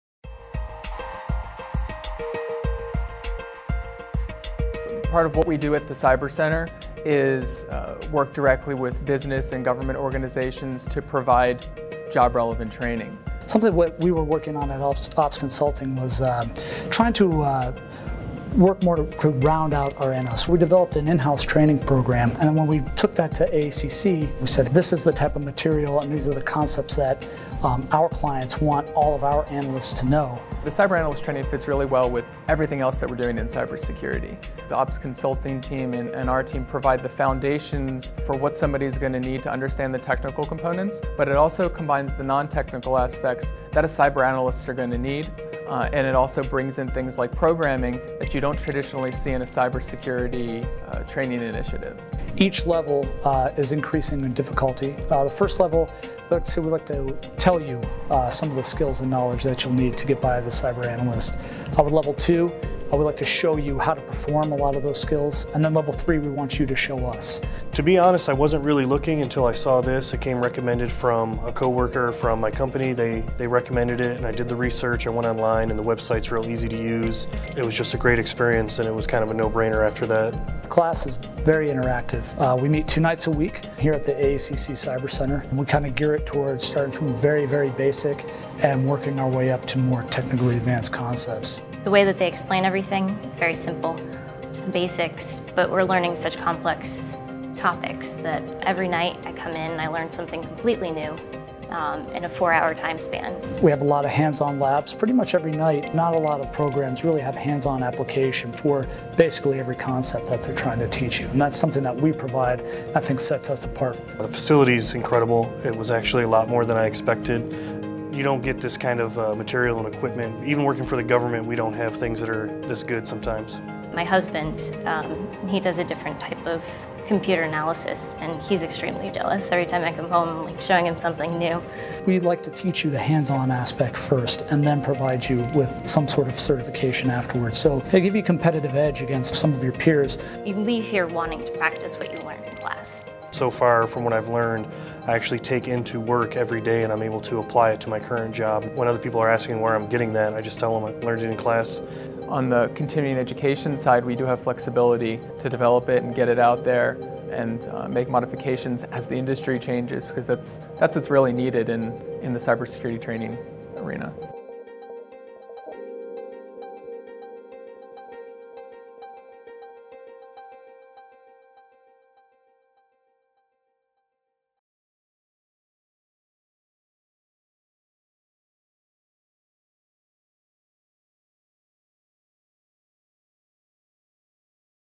Cyber Analyst Non Credit Training at AACC
The Cyber Analyst training is developed and delivered in collaboration with OPS Consulting, a company that specializes in intelligence and cyber analytics. The training is broken down into three levels of ascending difficulty that help participants gain knowledge, practice skills and demonstrate abilities.  The training is designed to bring the real world experiences of a cyber analyst to participants so that they are able to develop technical skills in the context of the job role.  The hands-on lab work reinforces the instruction and allows participants to apply their technical skills while also building critical thinking, data collection/analysis and communication core competencies.  Industry certification exam preparation for the CompTIA Network+, CompTIA Security+ and EC-Council Certified Ethical Hacker is also included in the training.